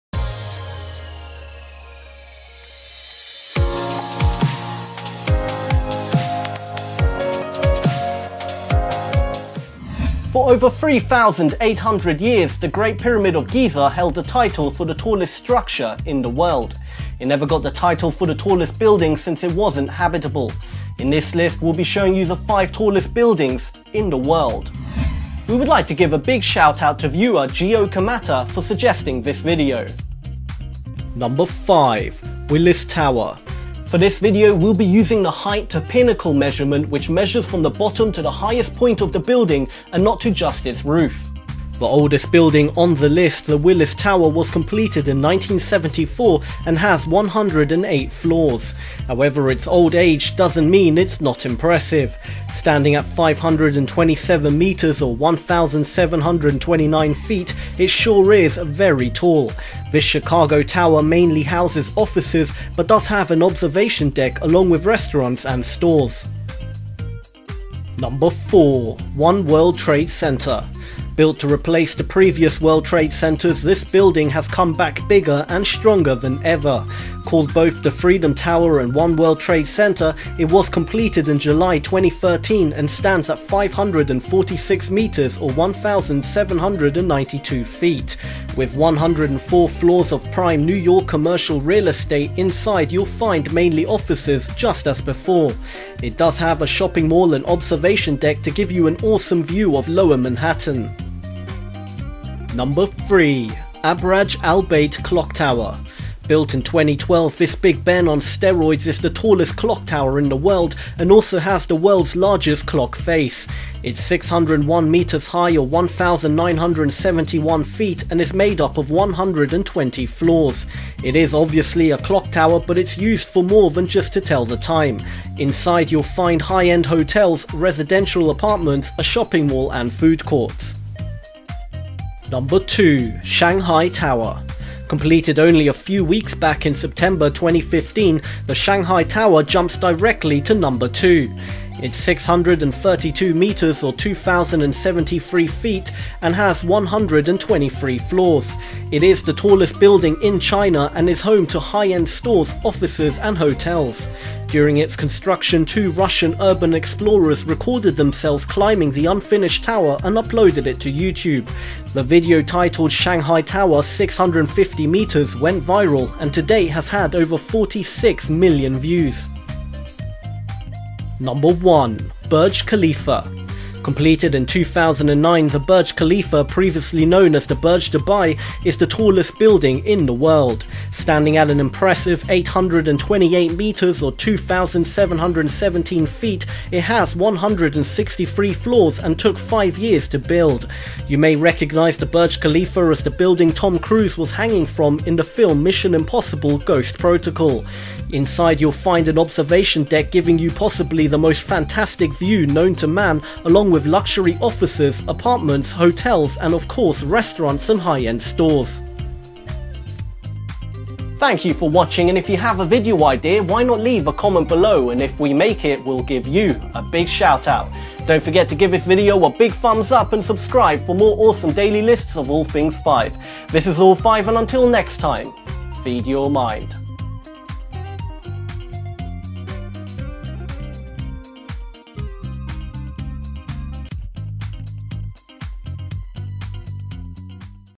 5 Tallest Buildings In The World!
For over 3,800 years, the great pyramid of Giza held the title as the tallest structure in the world.

It never held the title for the tallest building since it wasn't habitable.
In this list we'll be showing you the 5 tallest buildings in the world.

We would like to give a big shoutout to viewer Gyo Kamata for suggesting this video.

For this video we'll be using the Height to pinnacle measurement, which measures from the bottom, to the highest point of the building & not to just it's roof.

SECOND CHANNEL

5) Willis Tower

The oldest building on the list, the Willis Tower was completed in 1974 and has 108 floors.

However it's old age doesn't mean it's not impressive.

Standing at 527.0 m or 1,729 ft, it sure is very tall.

This Chicago tower mainly houses offices, but does have an observation deck along with restaurants & stores.

4) One World Trade Center

Built to replace the previous World Trade Centers, this building has come back bigger & stronger than ever.

Called both the Freedom Tower and One World Trade Center, it was completed in July 2013, and stands at 546 m or 1,792 ft.

With 104 floors of prime New York commercial real estate, inside you'll mainly find offices just as before.

It does have a shopping mall & observation deck to give you an awesome view of Lower Manhattan.

3) Abraj Al-Bait Clock Tower

Built in 2012 this Big Ben on steroids is the tallest clock tower in the world & also the has the world’s largest clock-face.

It's 601 m high or 1,971 ft, and is made up of 120 floors.

It is a obviously a clock tower but it's used for more than just to tell the time.

Inside you'll find high end hotels, residential apartments, a shopping mall & food courts.

2) Shanghai Tower

Completed only a few weeks back in September 2015, the Shanghai Tower jumps directly to spot two.

It's 632 m or 2,073 ft, and has 123 floors.

It is the tallest building in China, and is home to high end stores, offices, and hotels.

During it's construction, two Russian urban explores recorded themselves climbing the unfinished tower & uploaded it to YouTube.

The video titled Shanghai Tower (650 meters) went viral & to date has had over 46 million views.

1) Burj Khalifa

Completed in 2009 the Burj Khalifa, previously known as the Burj Dubai, is the tallest building in the world.

Standing at an impressive 828m or 2,717 feet, it has 163 floors and took 5 years to build.

You may recognize the Burj Khalifa as the building Tom Cruise was hanging from in the film Mission: Impossible – Ghost Protocol.

Inside you'll find an observation deck giving you possibly the most fantastic view know to man, along with luxury offices, apartments, hotels & of course restaurants & high end stores.

Until Next Time... Feed Your Mind!